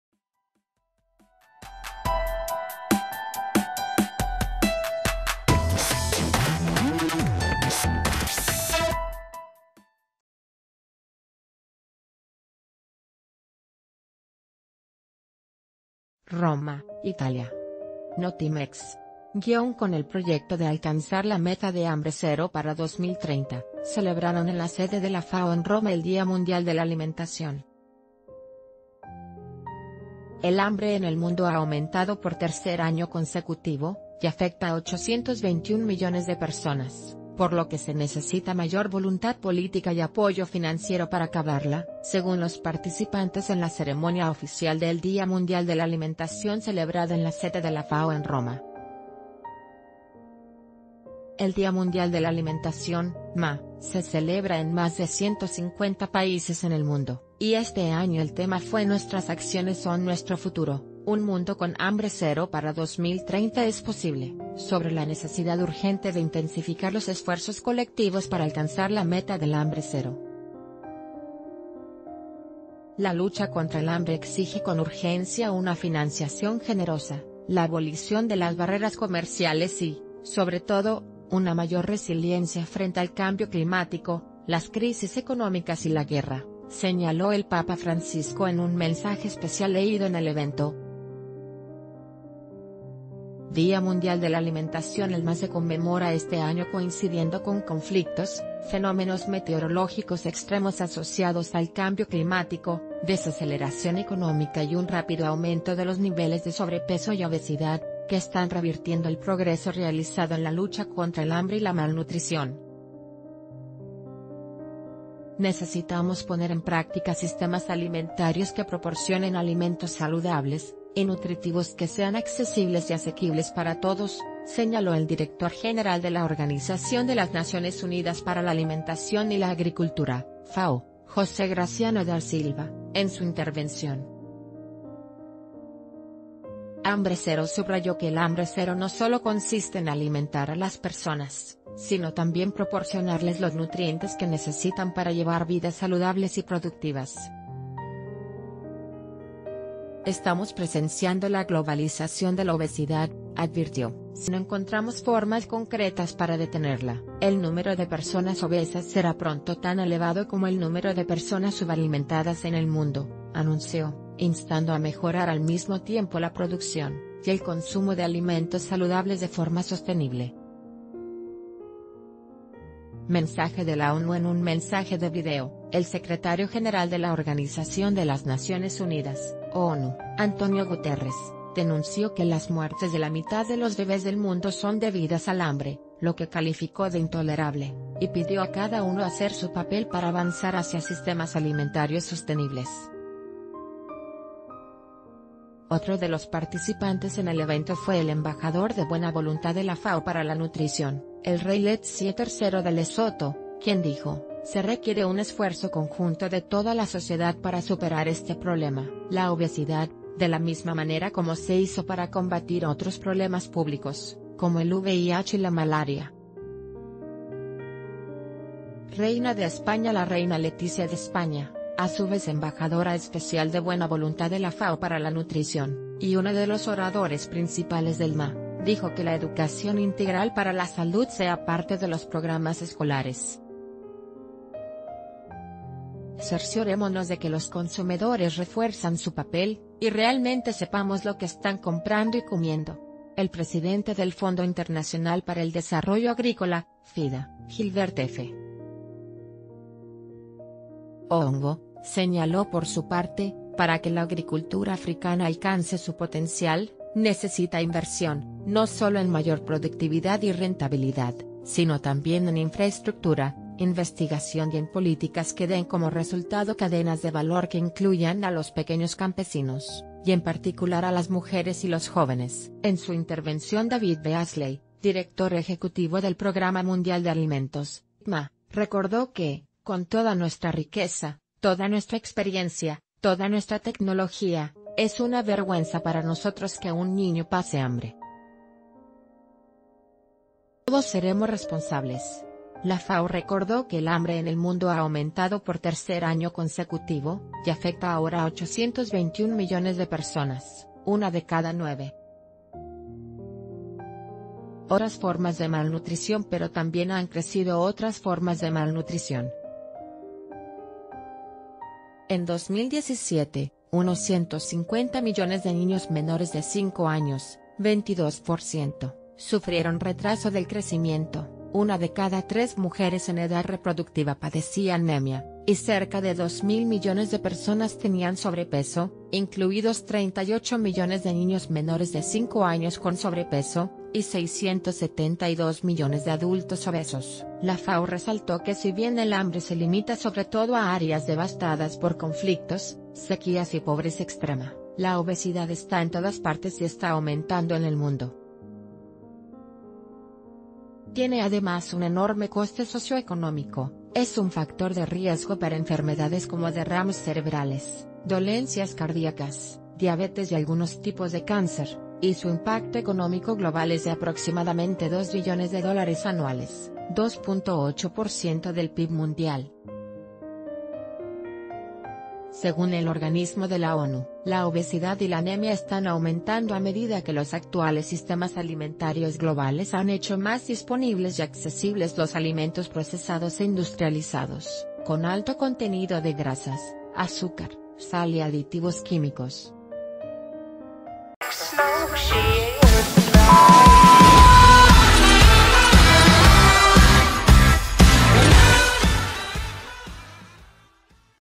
Hambre Cero para 2030, plantean en el Día Mundial de la Alimentación
ROMA, Italia.  (Notimex).- Con el proyecto de alcanzar la meta de Hambre Cero para 2030, celebraron en la sede de la FAO en Roma el Día Mundial de la Alimentación. El hambre en el mundo ha aumentado por tercer año consecutivo y afecta a 821 millones de personas, por lo que se necesita mayor voluntad...